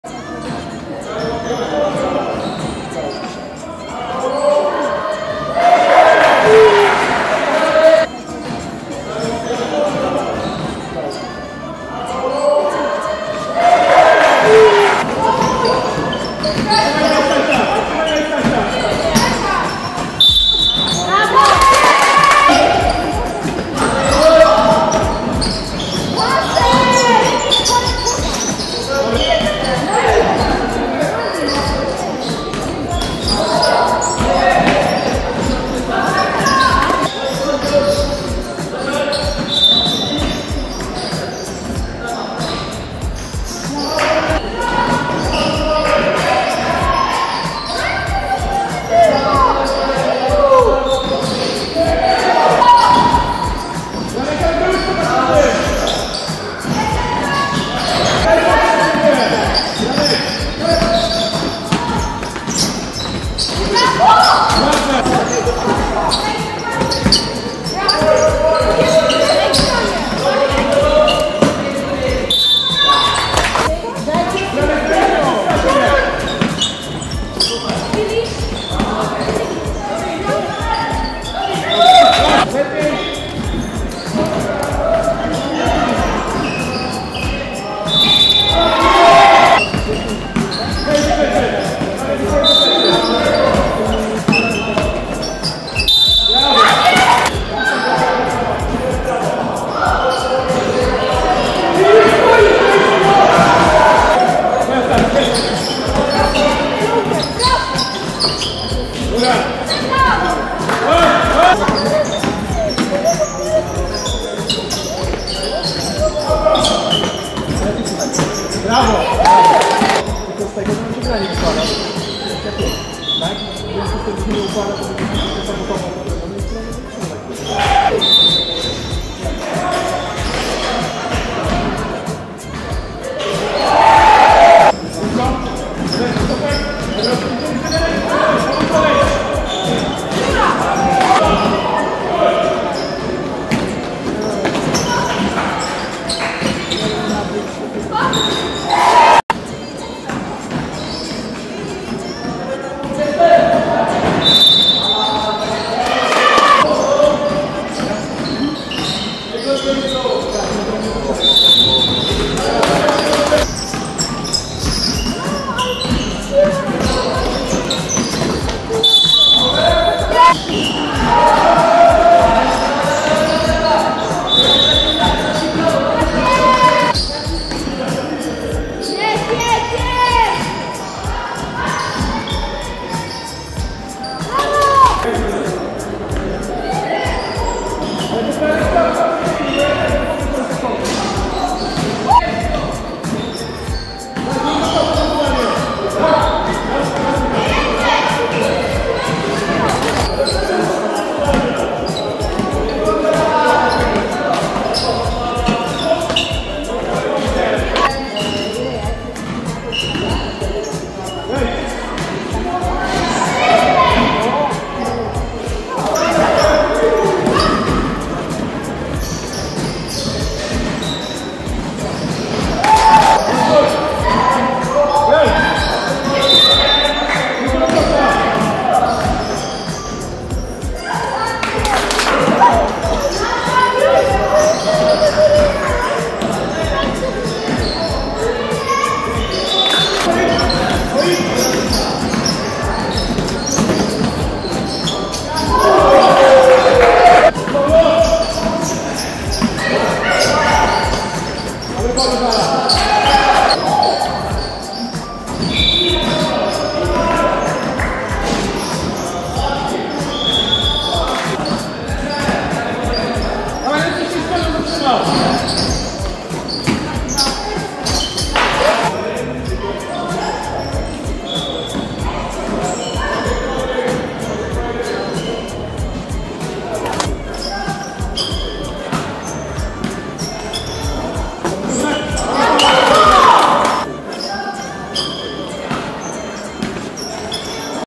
19 01 Szerszenie vs Byki – Dzika Liga ELITE 7
Szerszenie 61:52 Byki
M.Soczyński 19pkt 9zb 9stl
I.Białobrzeski 23pkt 14zb